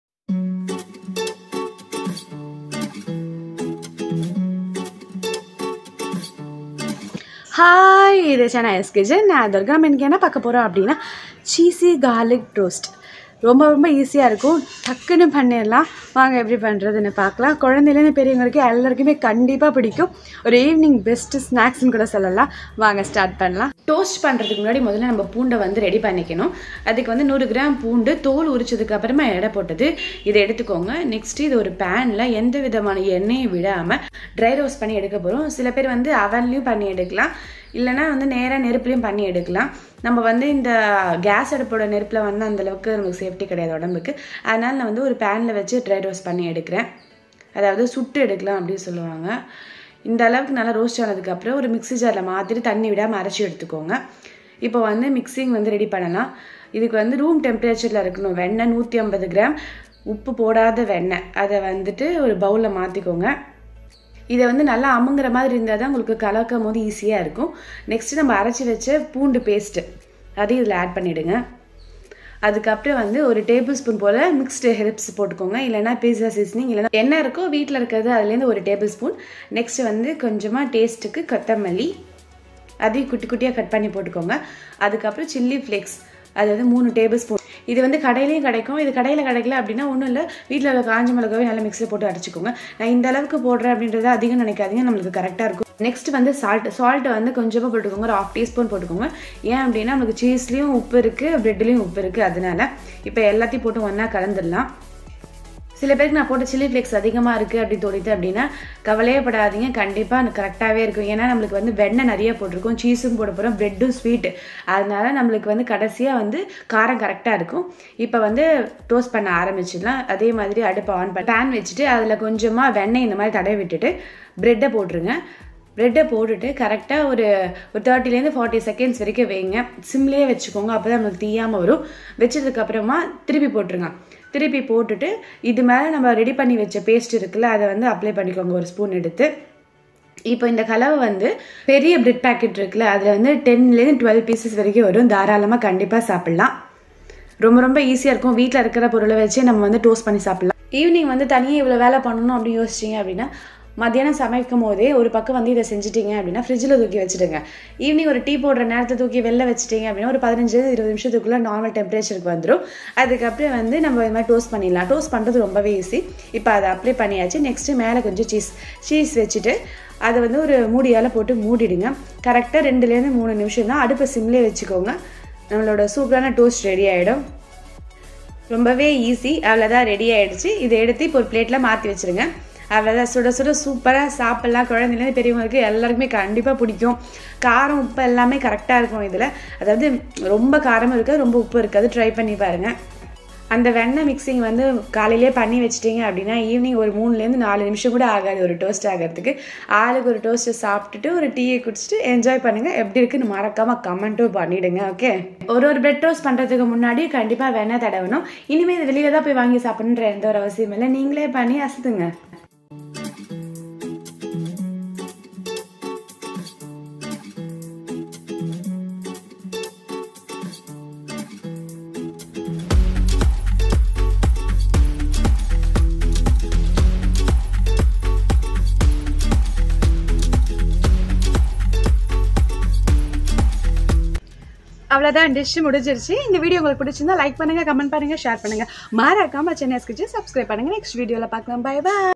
Cheesy garlic toast without oven | பிரட் பூண்டு டோஸ்ட் | garlic bread toast
One of the best and quick snack in the evening. Try it @chennaiskitchen_